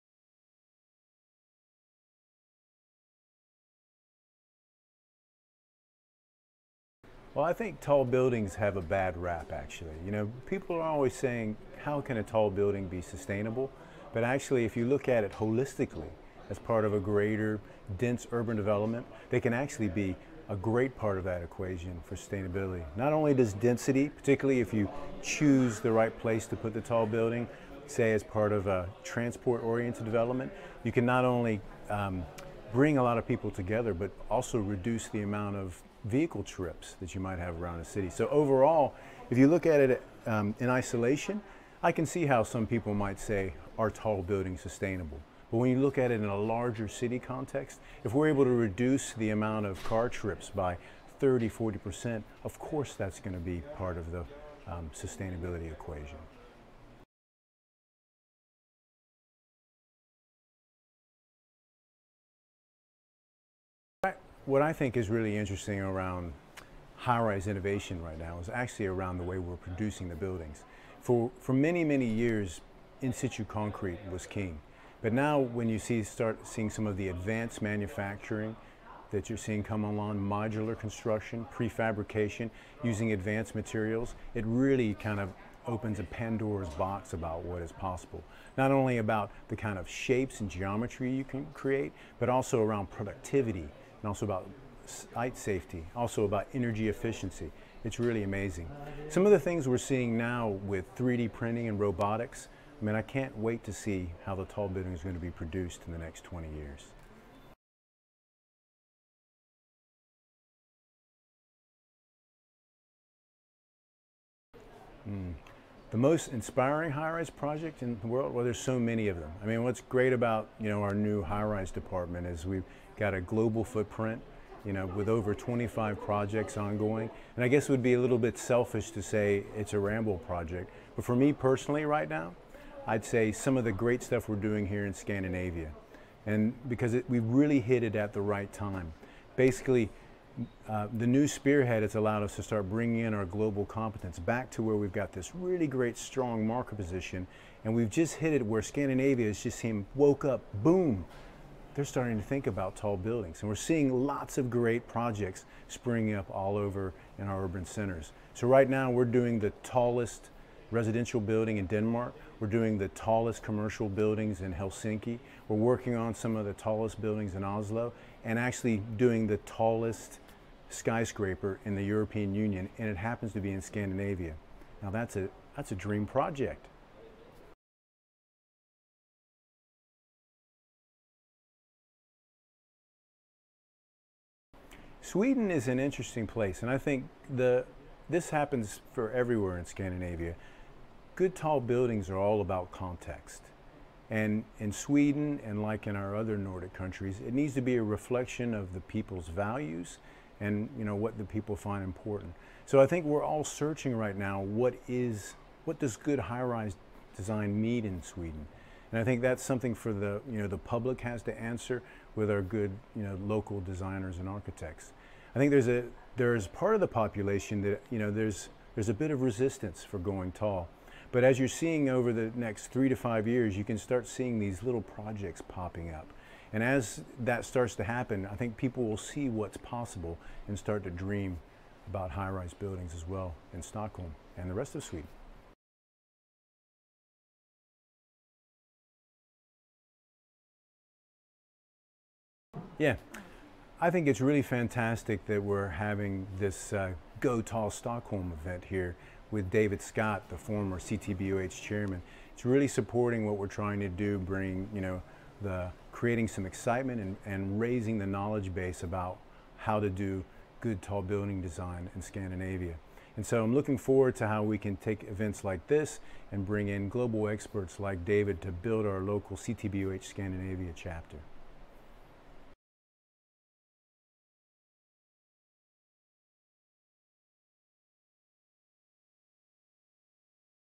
Go Tall Stockholm: intervju med Shonn Mills
Shonn Mills, Global High Rise Director på Ramboll om utvecklingen av höga byggnader i Sverige och Skandinavien och de möjligheter som kommer med det. Och finns det något som hämmar utvecklingen?

Från Go Tall Stockholm, ett seminarium om boomen för höga byggnader i Skandinavien, den 28 januari 2019.